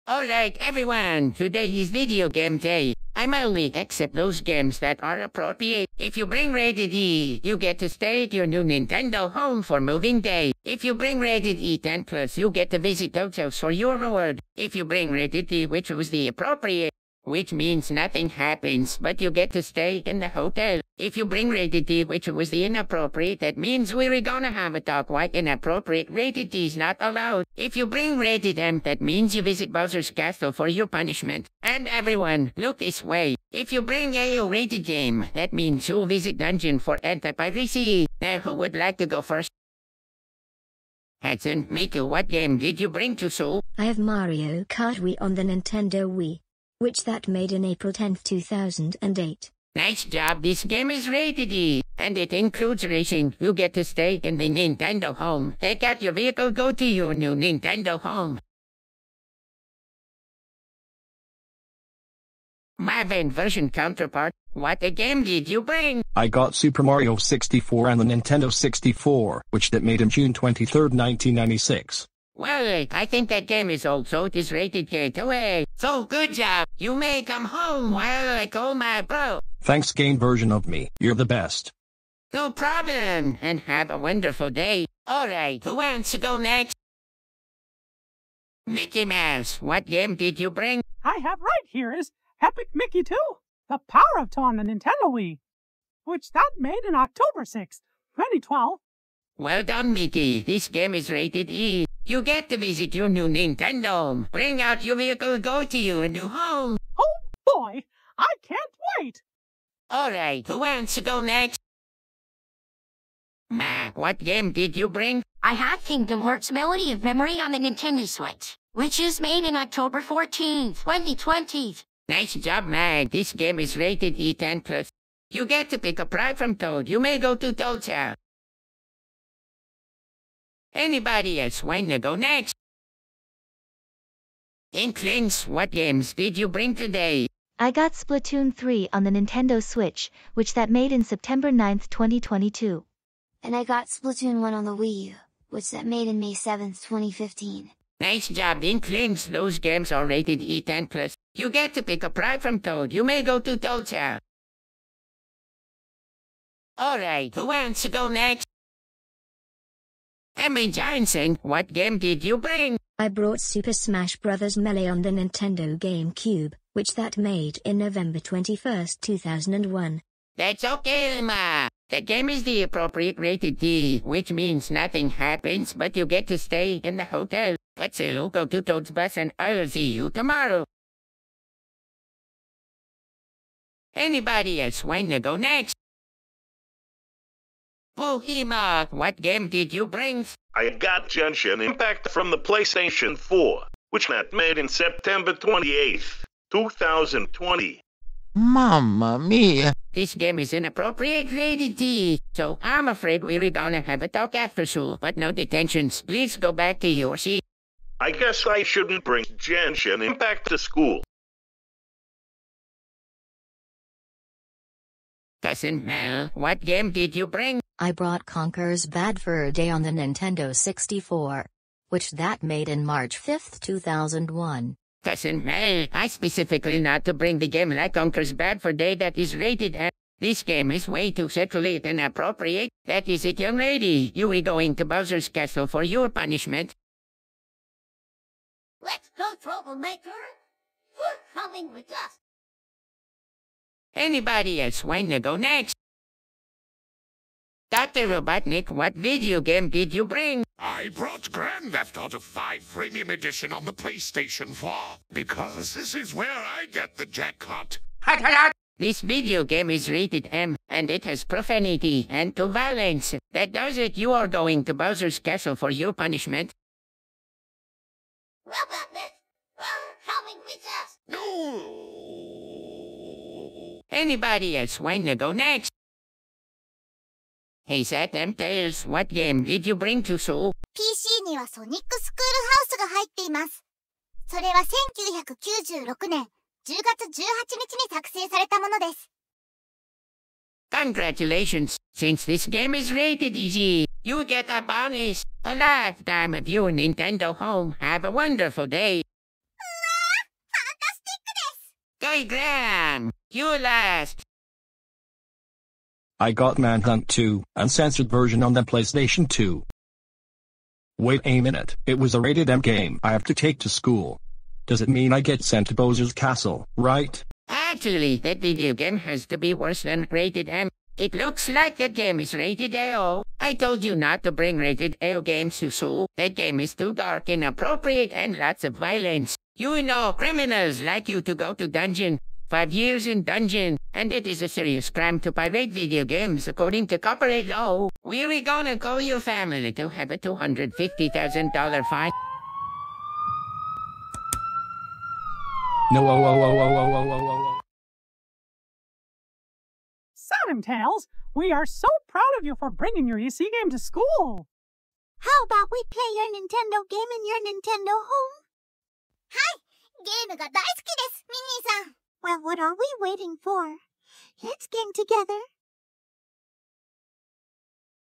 Video Game Day (Nintendo Edition) (Vyond Animation)
Special Credit To Charles Martinet, Wayne Allwine (RIP), Russi Taylor (RIP), Sean Marquette, Long John Baldry (RIP), Dolores Rogers, Haru Yosano & Voice Actors

OK, I Got Busy Playing Games Lately, So Now, I'm Gonna Do a Video Game Day (Nintendo Edition) At School On Vyond Animation. Now, I'm Gonna Use Luigi's Vyond Version Counterpart.

Now, I'm Gonna Use Japanese Voice Of SatAM Tails (SatAM Tails In Japan) From BunnyStudio @shiina_hekiru, @Aidencrossing, @SuperLuigi26TheMKTGamer2K5,  @allenelukito6373, @GalaxyPrincessScarlet1996, @ac_tycrossing2003, @xLunaa1, & @HighHonorsJacob, @与謝野はる.

It Was A Little Cringe, I've Apologize.

Use Full Screen.

Note:  Luigi's Vyond Version Counterpart Is Steven, Not For Nintendo Character For Me.

Only Characters That Are The Letters & Words For Uberduck, Or FakeYou.

K-A Is Included "Kay To Ay"
EC Is Included "Ee See"
E Is Included "Ee"
E10+ Is Included "Ee 10 Plus"
Appropriate & Inappropriate Rated T Is Included "Approapriate & Inapproapriate Rated Tee"
M Is Included "Em, Or Emm"
AO Is Included "Ay Oh"
Pirate Is Included "Py Rate"

Luigi: Himself (Charles Martinet)

Hatsune Miku & Emma Johnson: Emma

Luigi (Vyond Version Counterpart):

Mickey Mouse: Himself (Wayne Allwine)

Mac (foster's home for imaginary friends): Himself (Sean Marquette)

Inklings: Amber (Girl) & Justin (Boy) TTS

Booheemoth: Wiseguy. TTS

Cousin Mel: Kimberly TTS

Bowser Jr: Himself (Dolores Rogers)

Dr. Robotnik: Himself (Long John Baldry)

SatAM Tails (SatAM Tails In Japan): @shiina_hekiru, & @与謝野はる

Guy Graham (Vyond Version): Steven TTS

Minnie Mouse: Herself (Russi Taylor)